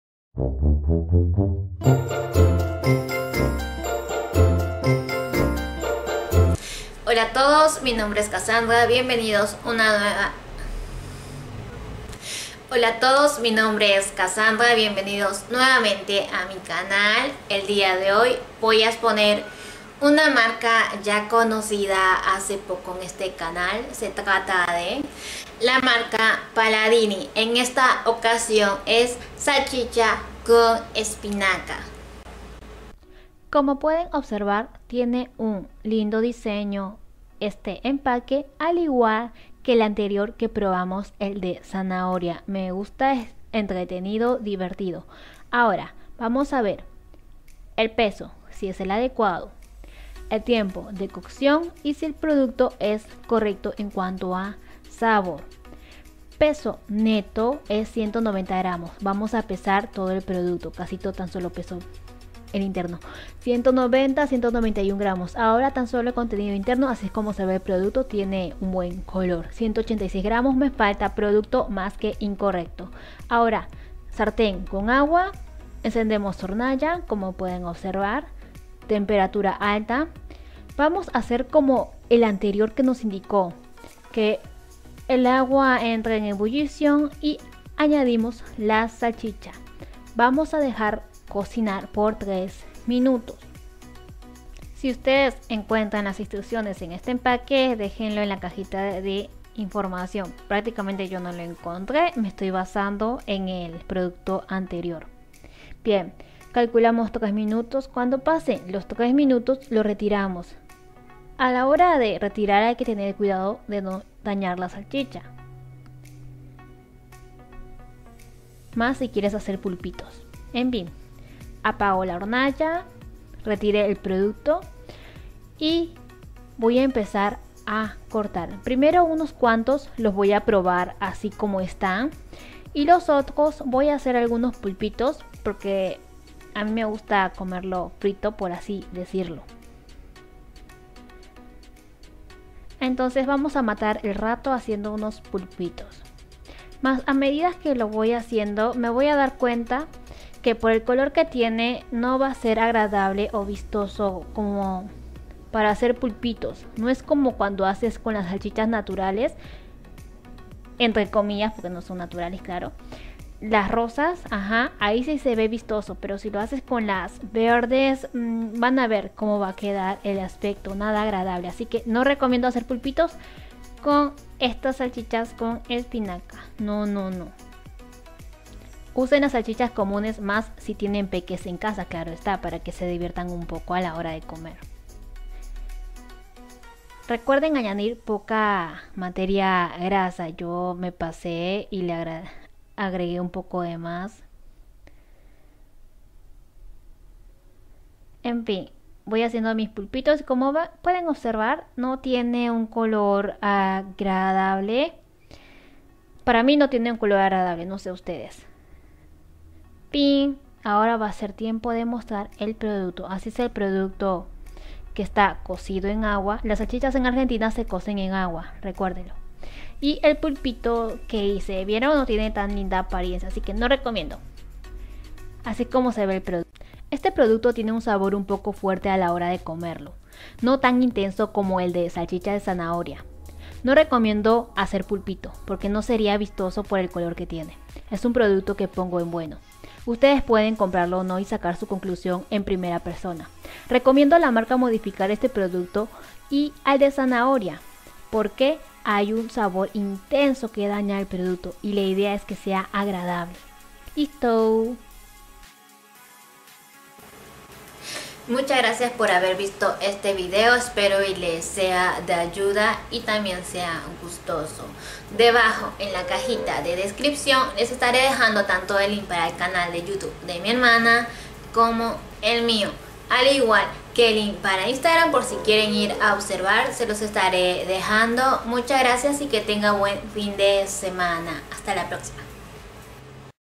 ¡Salchichas con espinaca-Paladini!
En ésta oportunidad probamos uno de los productos de la marca Paladini.

Calificación: Bueno.

NOTA: Al igual que el otro producto(Salchicha con zanahoria) este tiene un sabor un poco fuerte, el cuál no debería tenerlo. Tendría que modificar eso, ya que es el único detalle en contra.

Si ustedes quieren probarlo y sacarse la duda en primera persona pueden hacerlo.

Lo que yo busco en cada producto es si cumple con la cantidad exacta del producto, también si la forma de preparación(instrucciones) son correctas o no.

En cuánto a sabor doy mi opinión básica, puede ser:

-Muy bueno(Lo compraría).
-Bueno(Puede que lo vuelva a comprar o no).
-No rico(No lo compraría).

•Ya lo probaron?
• Lo recomendarían?